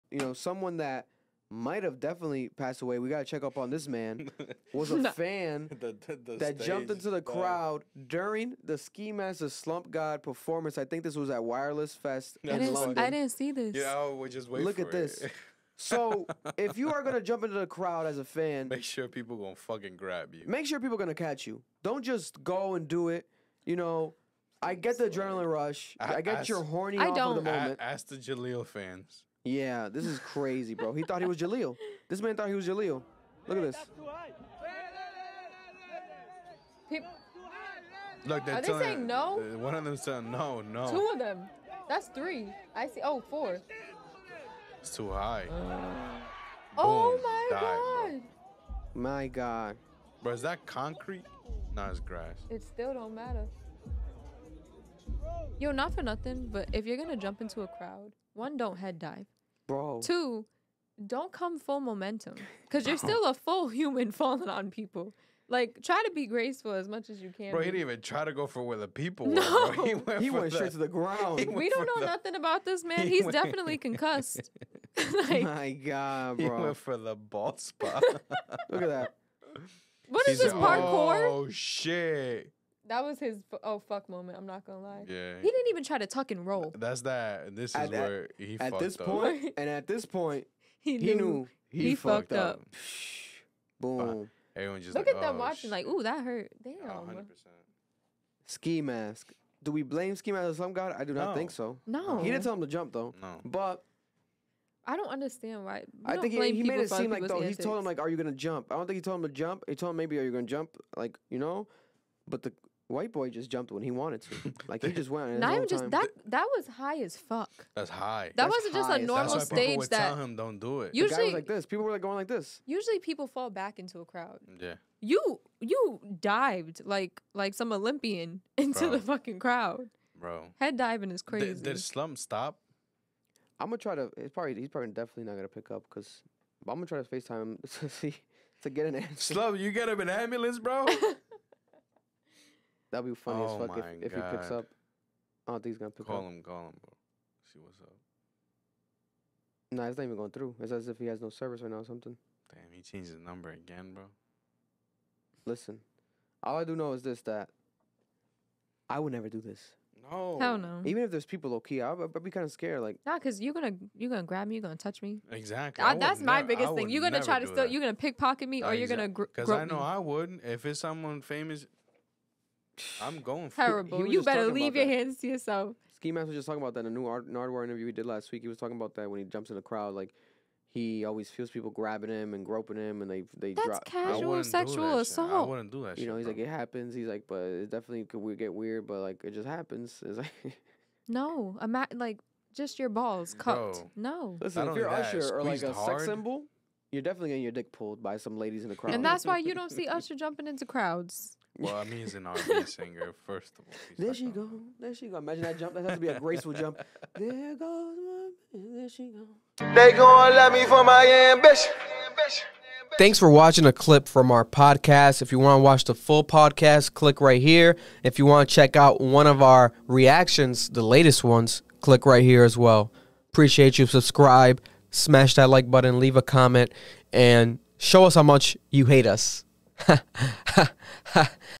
Ski Mask The Slump God Fan Dives Face First Into Crowd During Concert
Want a promo snippet of your song at the beginning of the video?

RIP: Nipsey, Mac, XXXtentacion, Juice, Pop, Von, DMX, Virgil, Dolph, Takeoff